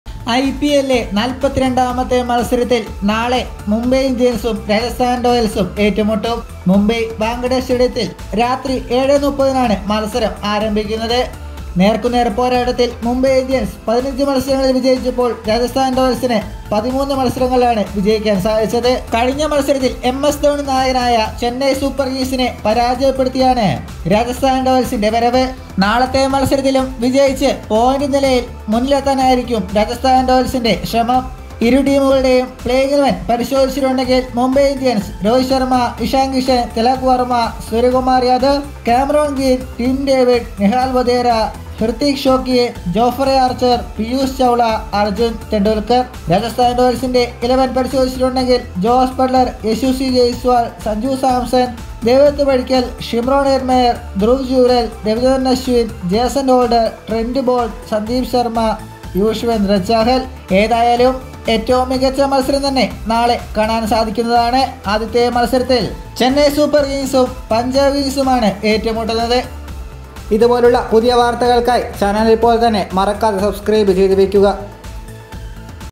ജയിച്ചാൽ ടേബിൾ ടോപ്പേഴ്സ് സഞ്ജു ടീം നാളെ ഇറങ്ങുന്നു 💥|RR VS MI PLAYING 11 | SANJU SAMSON
SANJU SAMSON